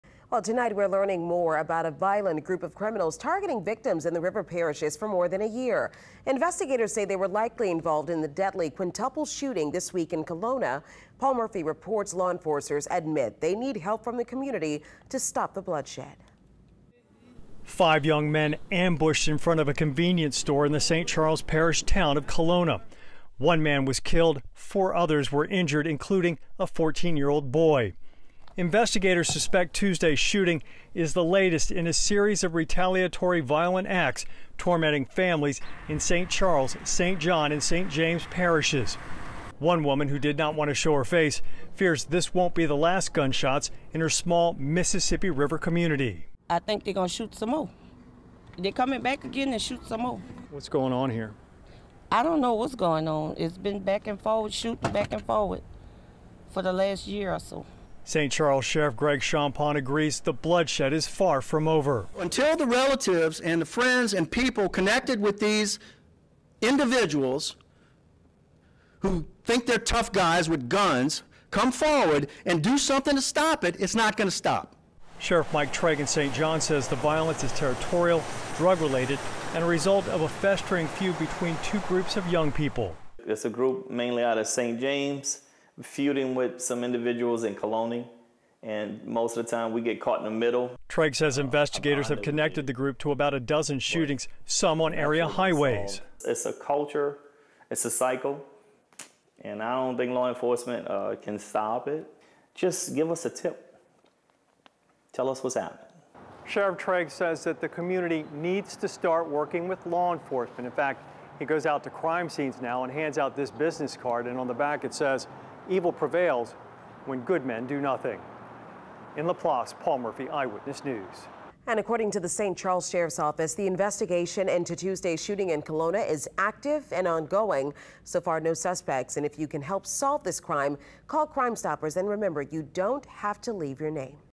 River Parish sheriffs call for public's help to end cycle of violence
Five young men, ambushed in front of a convenience store in the St. Charles Parish town of Killona.

One man was killed, and 4 others were injured including a 14-year-old boy.

Investigators suspect Tuesday’s shooting is the latest in a series of retaliatory violent acts, tormenting families in St. Charles, St. John and St. James Parishes.

One woman who asked not to be identified, fears this won’t be the last gunshots in her small Mississippi River community.

“I think they’re going to shoot some more,” she said. “They’re coming back again to shoot some more. I don’t know what’s going on. It’s been back and forth shooting, back and forth for the last year or so.”